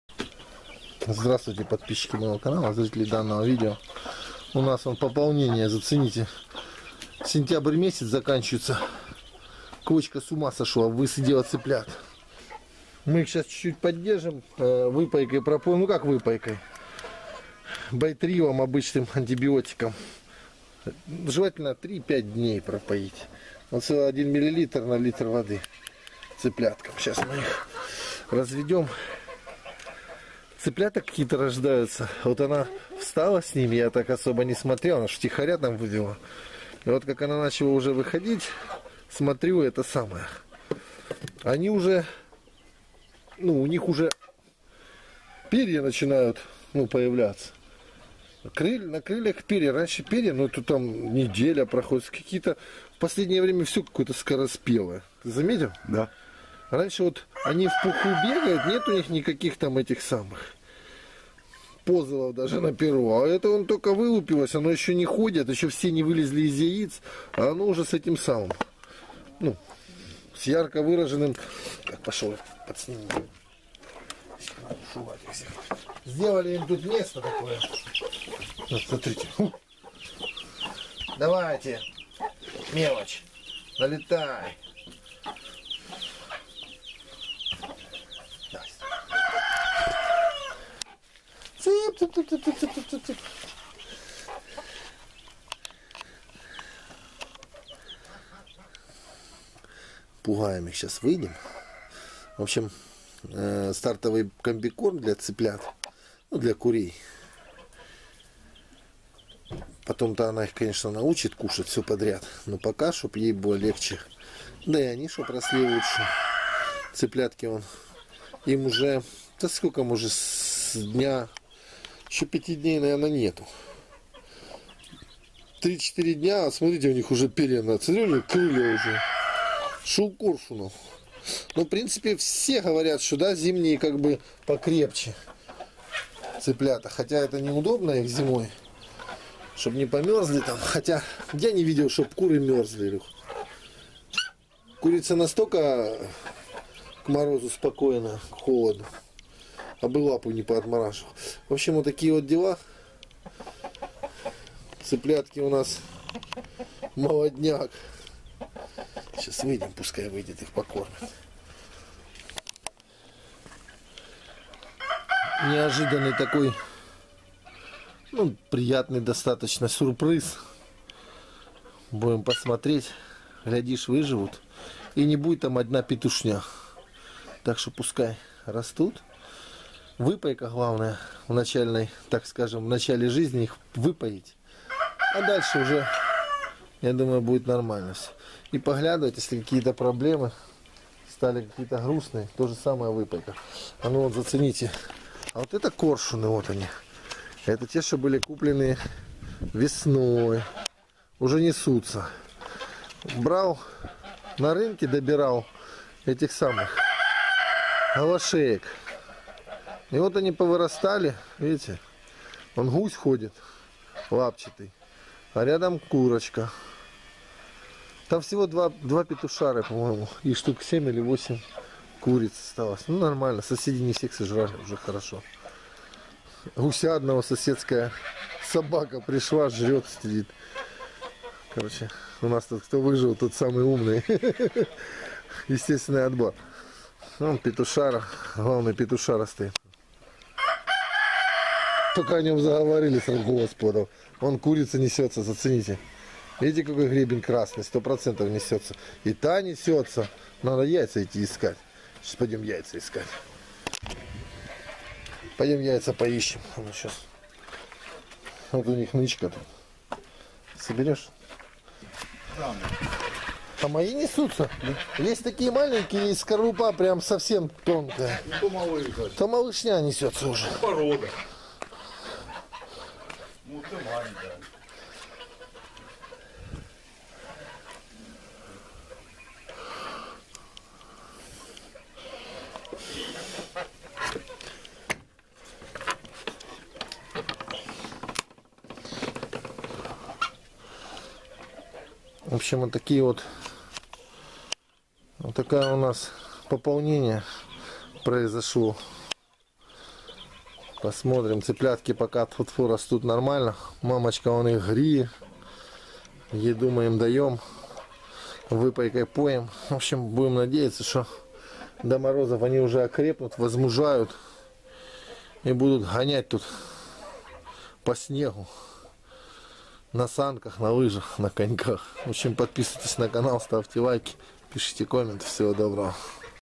Наседка с цыплятами новогоднее пополнение .
На дворе уже осень но наседка высидела цыплят и они вырастут к новому году ,если конечно ничего им не помешает.Не для кого не секрет,что цыплят обычно покупают или высиживают весной ,что бы летом было проще их выращивать ,но многие говорят,что зимние цыплята гораздо сильнее весенних,так ли это скоро узнаю и я.

It’s already autumn in the yard, but the hen has hatched the chickens and they will grow up by the new year, unless, of course, nothing interferes with them. It’s not a secret for anyone that chickens are usually bought or hatched in the spring, so that it would be easier to grow them in summer, but many say that winter chickens are much stronger than spring ones, so I will soon find out.